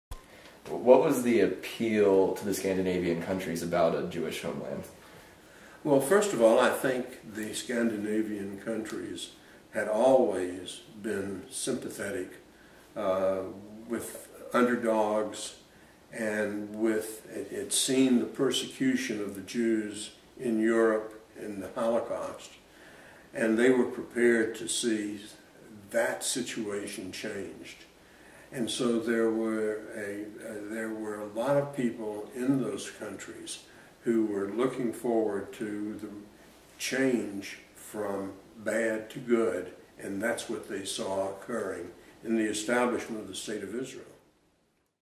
Interview with Former Congressman Elliott Levitas p5
National History Day - David Liss and Patricia Chang